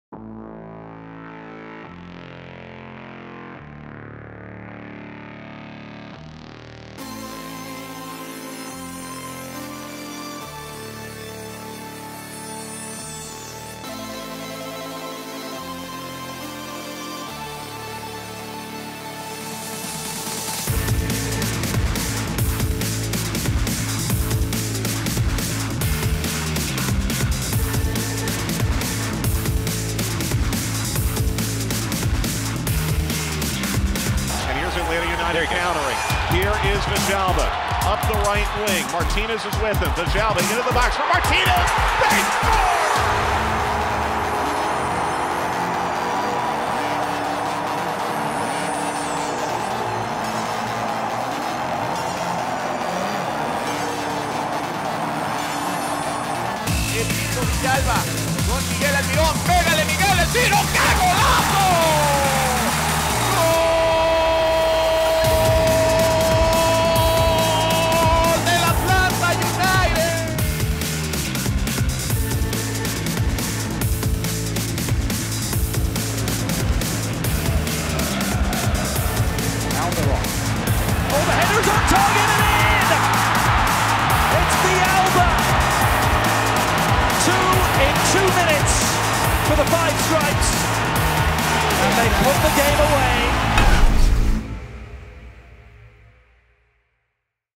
Nonstop Access: Atlanta United 3-1 D.C. United
In the march and from the field. A memorable night for Atlanta United's first win of 2018.